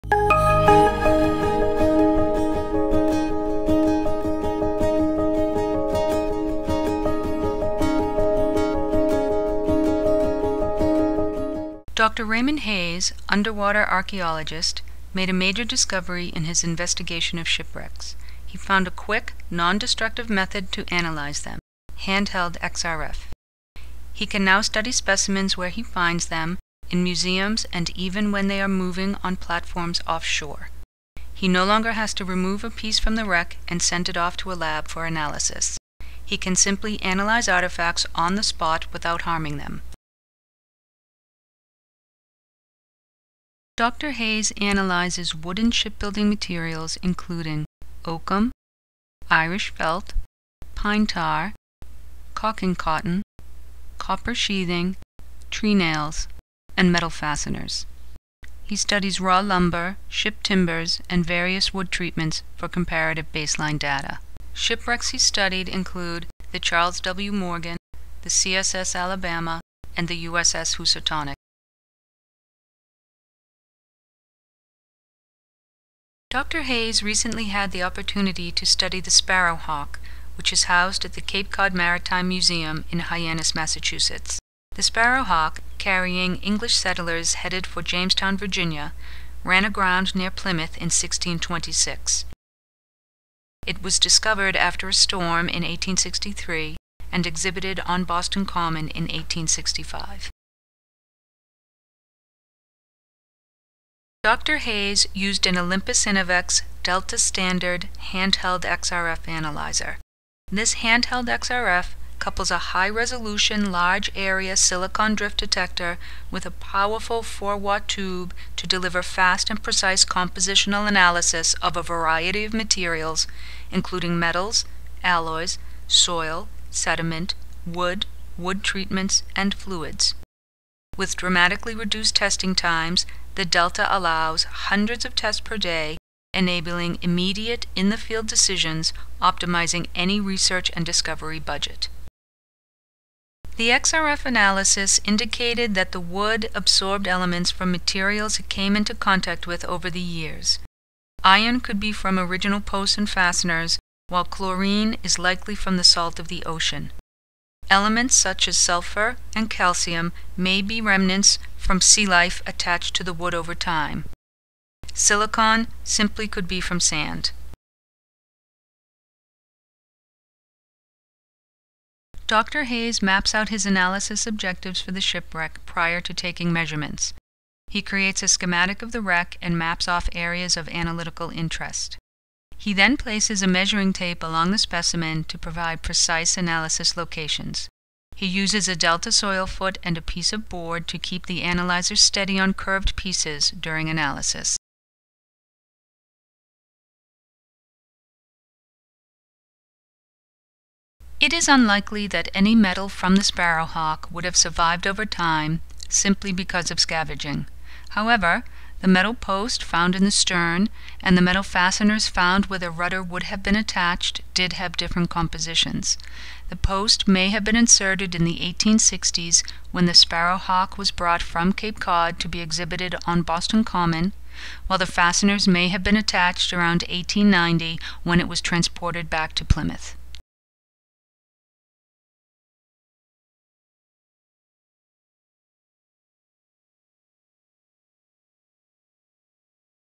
Handheld XRF for Shipwrecks
Dr. Raymond Hayes discovered a quick, nondestructive method to analyze shipwrecks, an Olympus DELTA Handheld XRF. Most recently, he studied the Sparrow Hawk, a ship that ran aground near Plymouth in 1626 while carrying English settlers headed for Jamestown Virginia.

Questions? Contact an Olympus Specialist